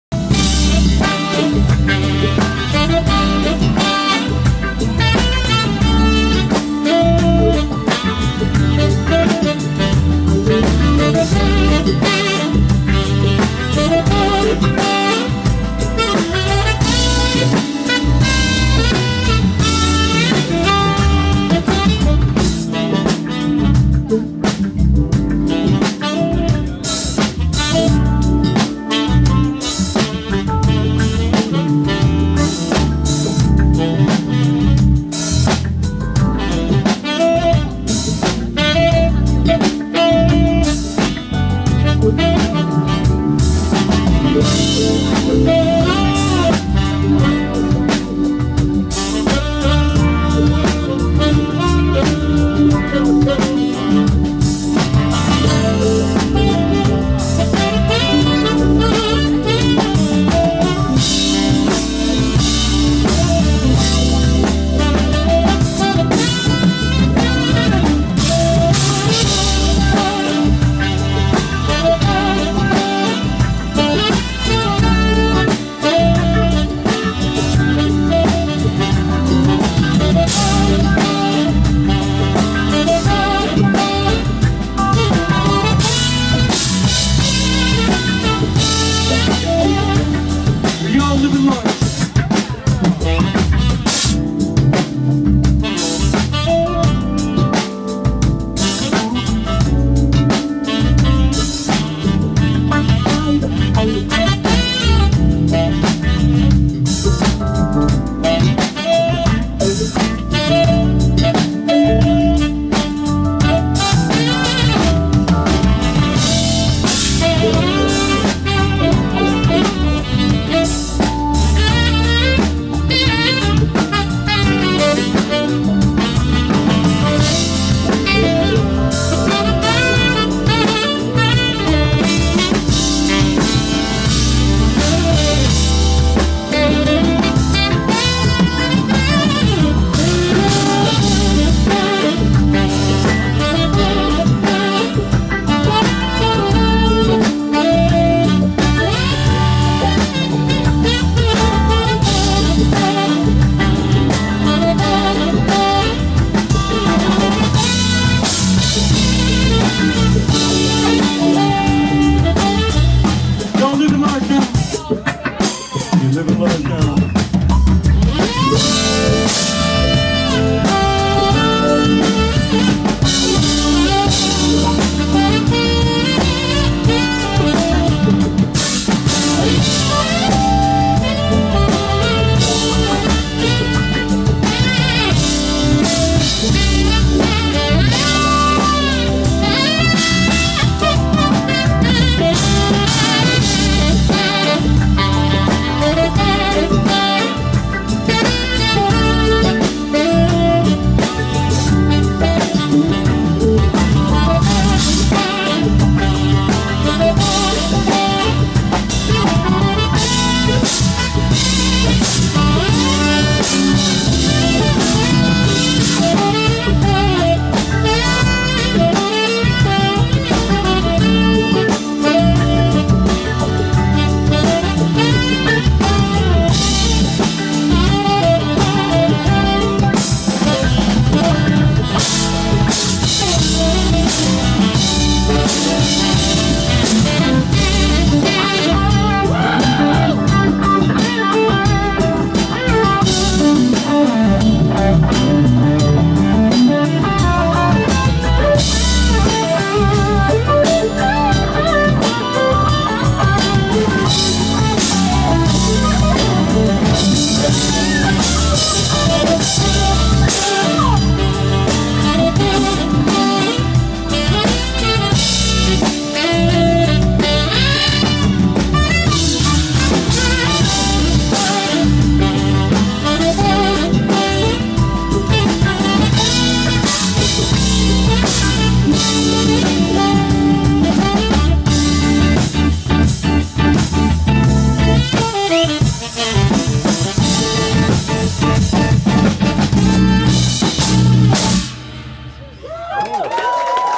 Euge Groove at PizzaExpress Jazz Club - Holborn - London - 24 Nove 2017
Band Lineup
Euge Groove (sax)
Luke Smith (keyboards)
Mark Jaimes (guitar)
Orefo Orakwue (bass)
Andrew Small (drums)